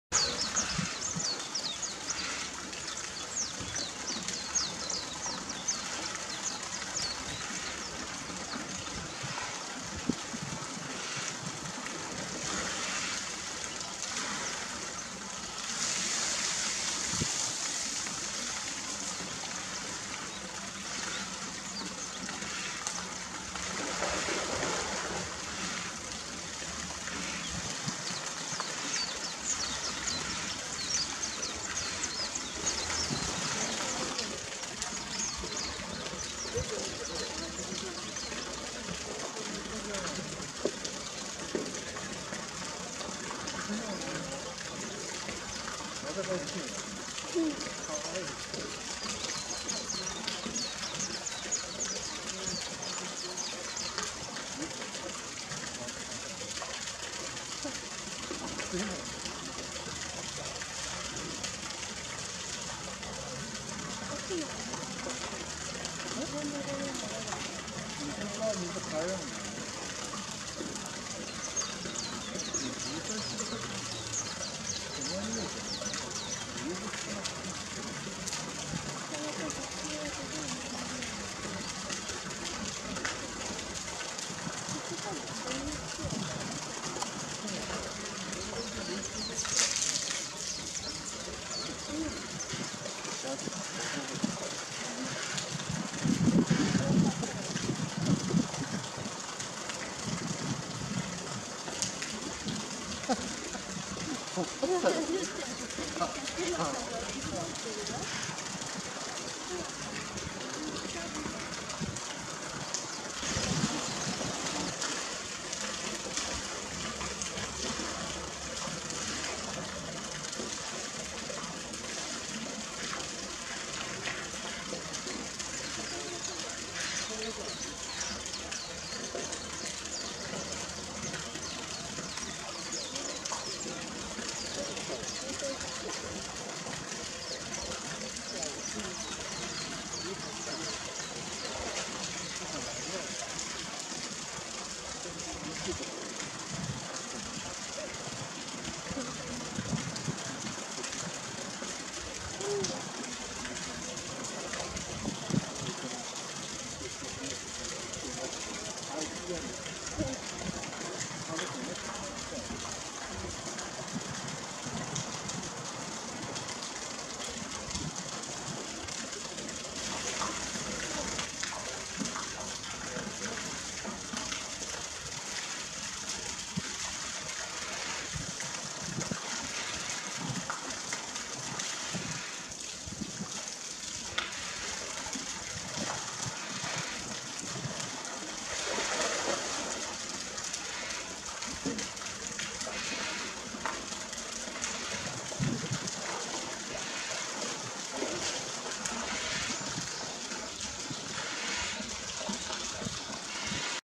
アマナちゃんのおとなしい水浴び
2019年5月30日、のんほいパークのエゾヒグマのメス、アマナ。（ブログ説明用）